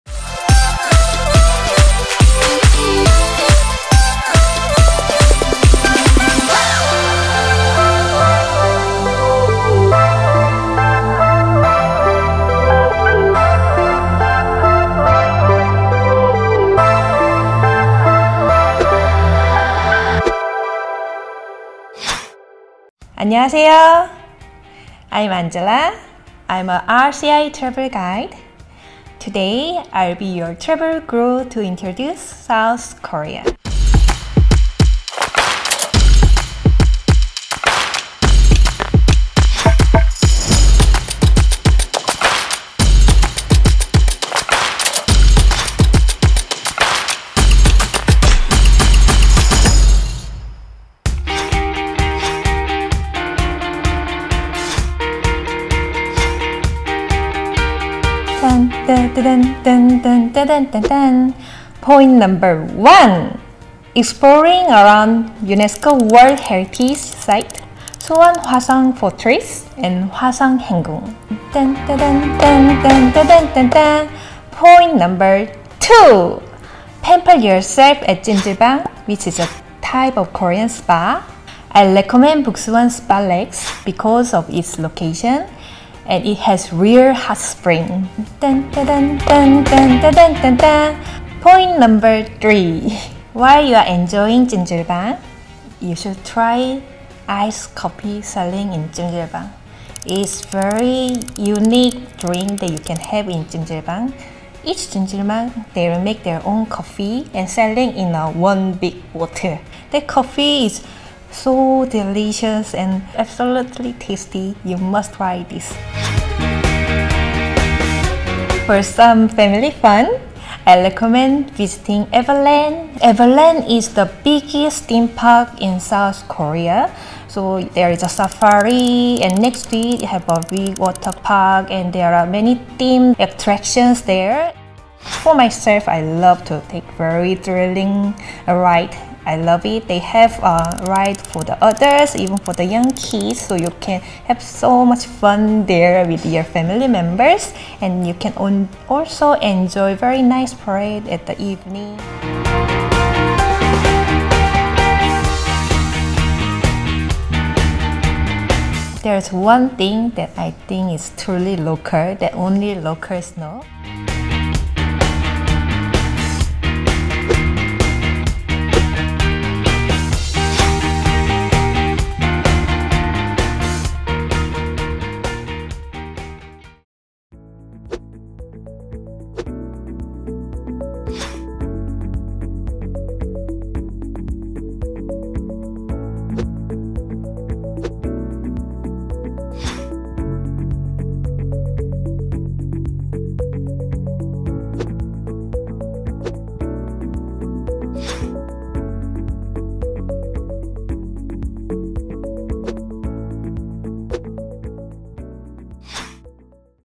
Episode 5.1 Say "Annyeonghaseyo" to Angela in South Korea
RCI Travel Guru Series Thrill Voyager will take us the Land of the Morning Calm and her own hometown, South Korea. In her first episode in the series, Angela shares the must dos and visits in the K-world. Come and let's explore together as we unravel abundant tourist attractions, mesmerizing sights and K-secrets only from the K-expert, Angela. Buckle up and watch this last three episodes of the RCI Travel Guru Series.